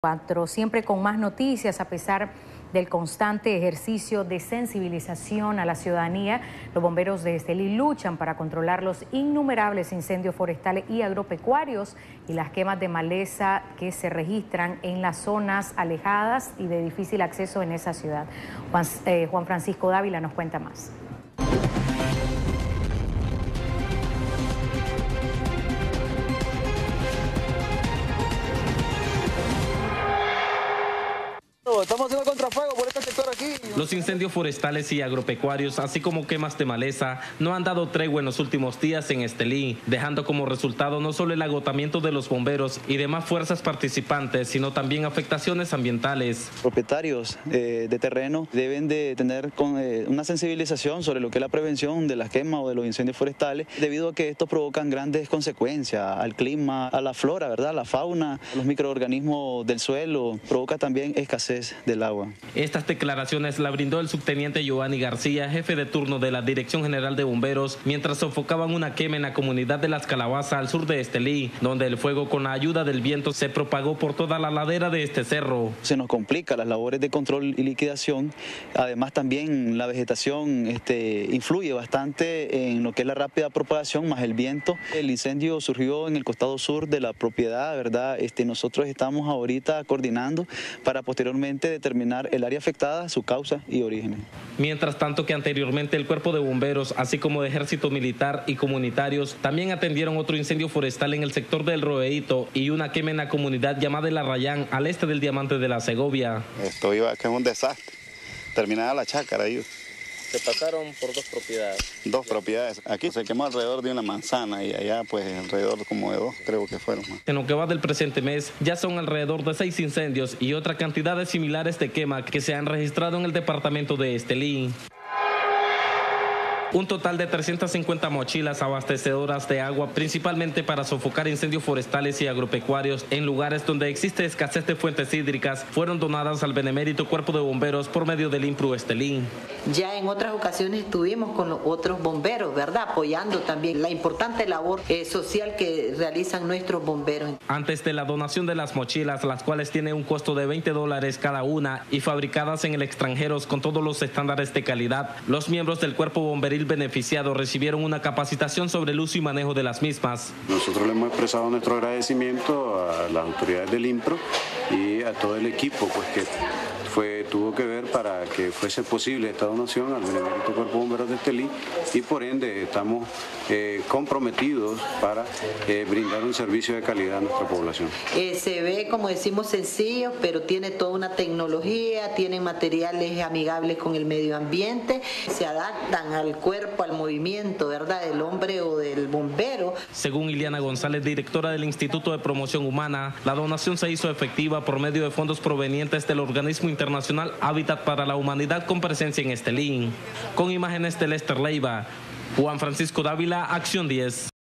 Noticias Departamentales
A pesar del constante ejercicio de sensibilización a la ciudadanía, los bomberos de Estelí luchan para controlar los incendios forestales y agropecuarios, que se registran en esa zona.